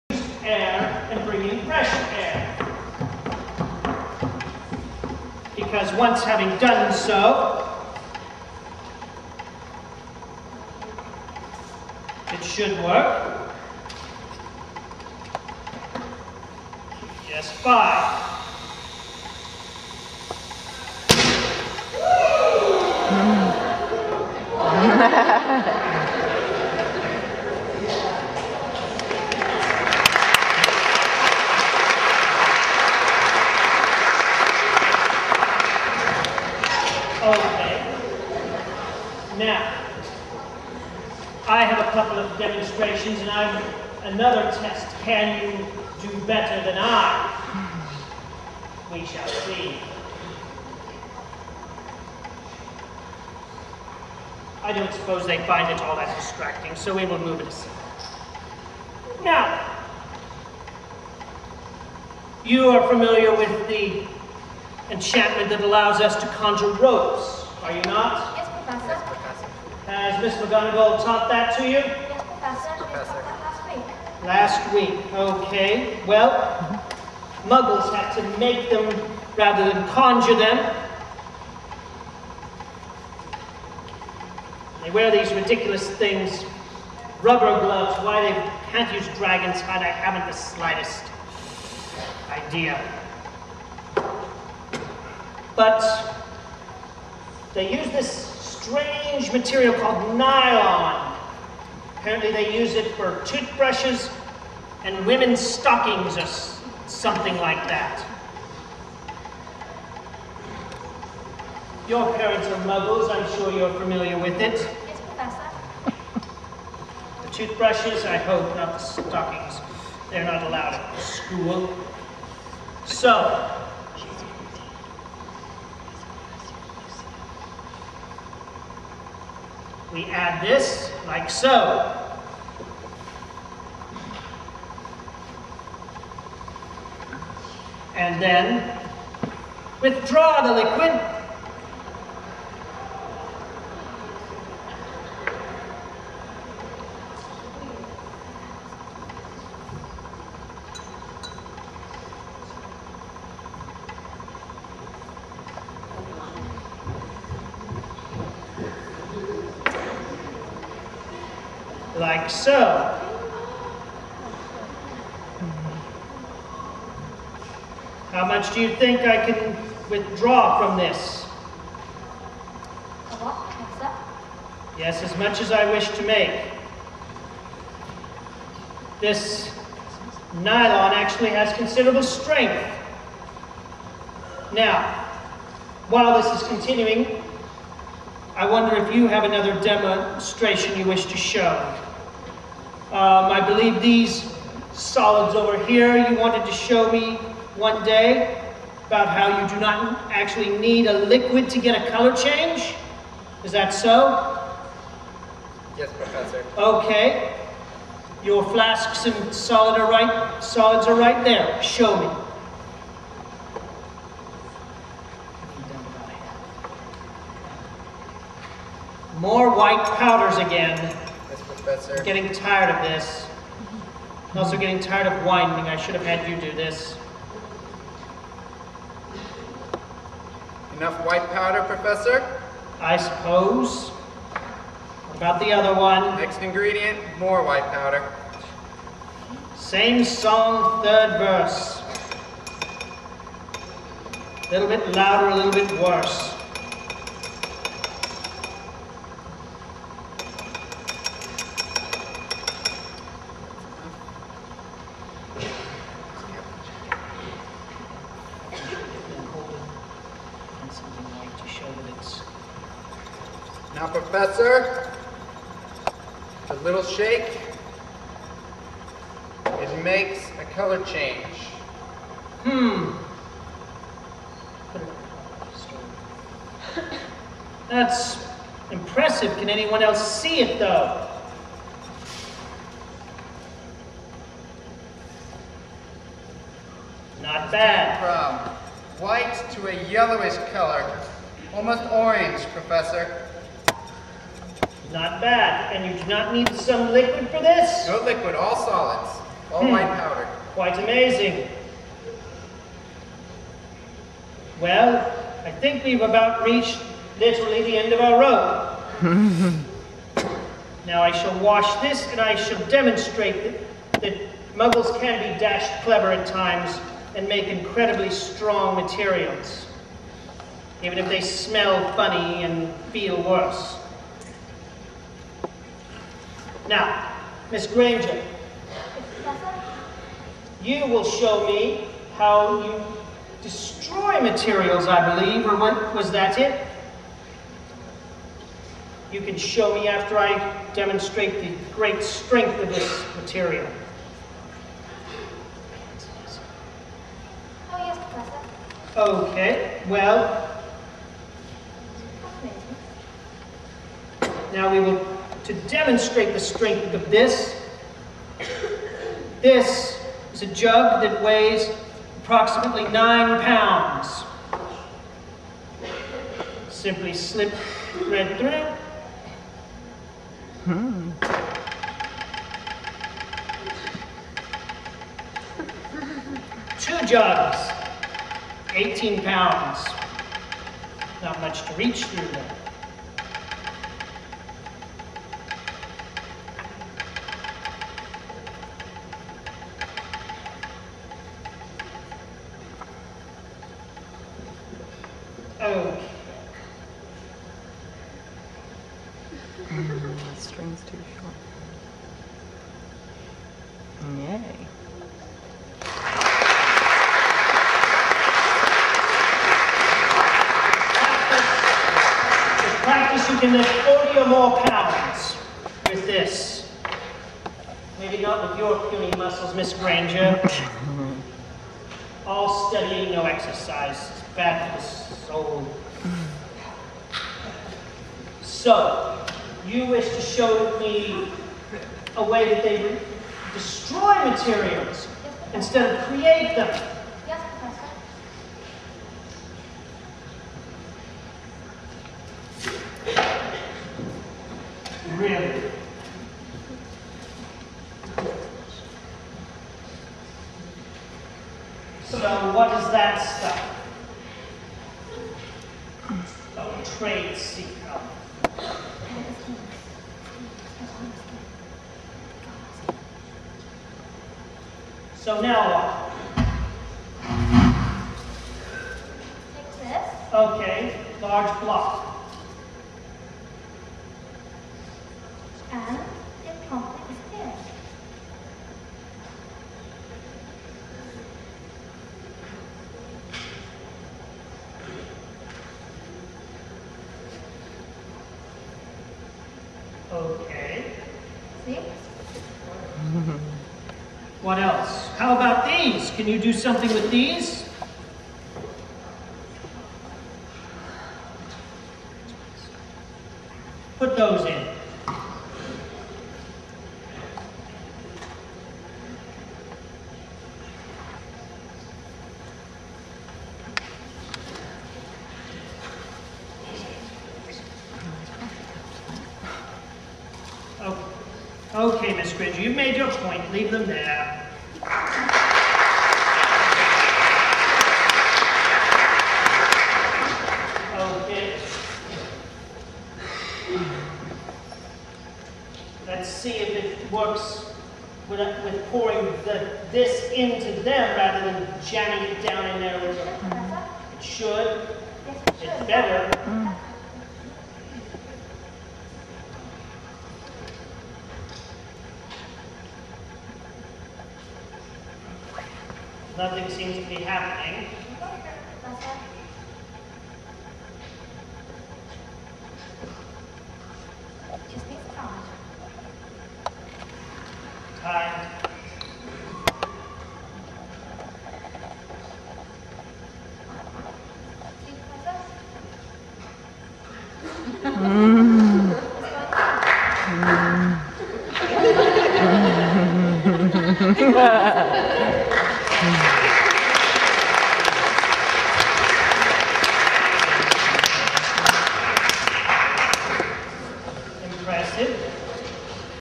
Omniplex Harry Potter Chemistry Show Part 3
Mr. Jordan and Miss Ferguson had to prove by means of Chemistry that they were able to perform magic (chemistry) to Dr. Ferguson. Watch and Enjoy.